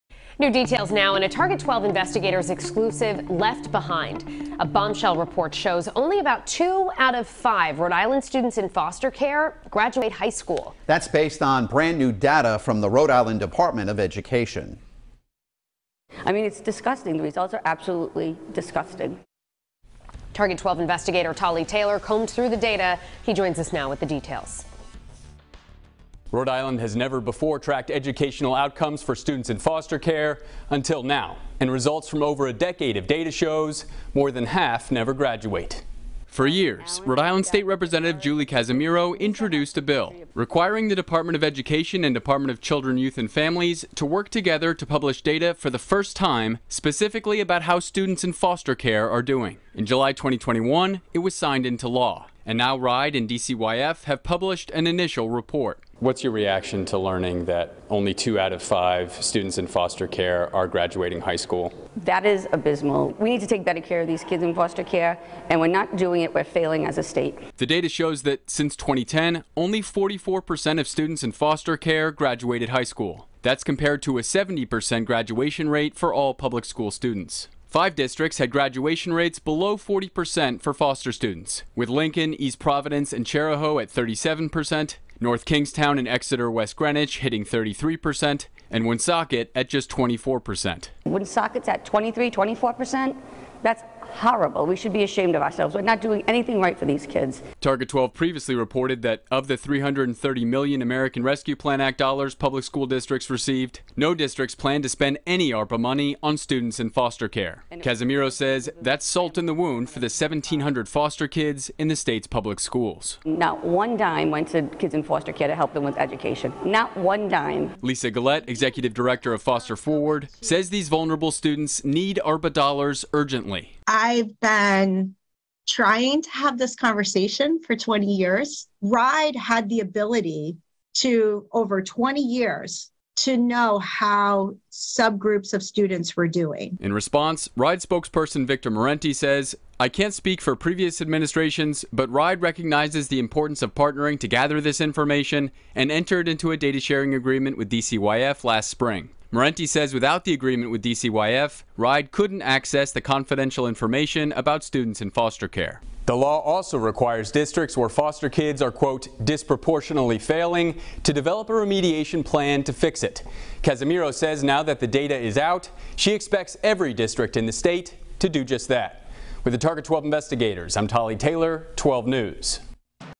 Target 12: Left Behind
Only 44% of Rhode Island K-12 students who have been in foster care graduated high school between 2010 and 2022, according to first-of-its-kind data from the R.I. Departments of Education and Children, Youth and Families.